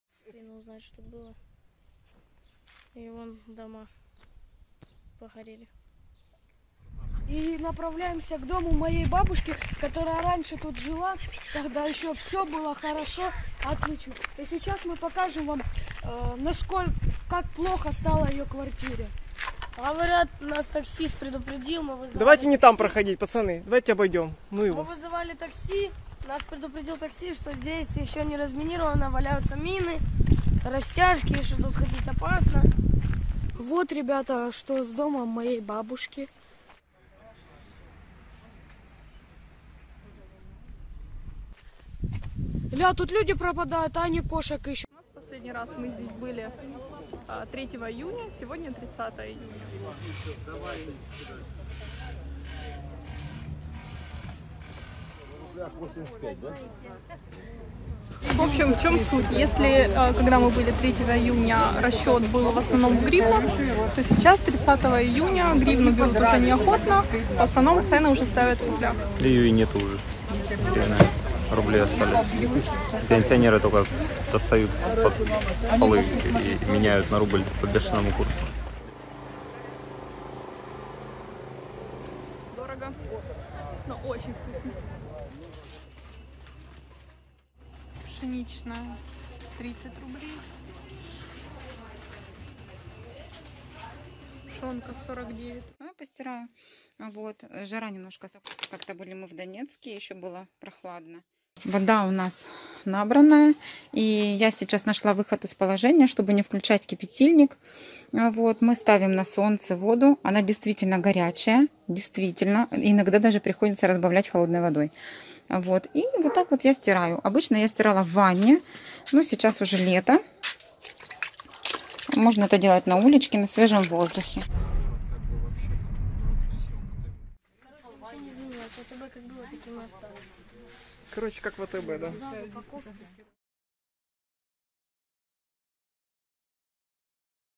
Video blogovi dokumentuju svakodnevnicu u okupiranoj Ukrajini
Dječak pokazuje izgorjeli dom svoje bake u postu na blogu iz okupiranog Mariupolja, dok žena govori kako ruska rublja zamjenjuje ukrajinsku grivnju u većini transakcija. Brojni videoblogeri dokumentuju svakodnevni život nakon što su ruske trupe izvršile invaziju i preuzele kontrolu nad gradom.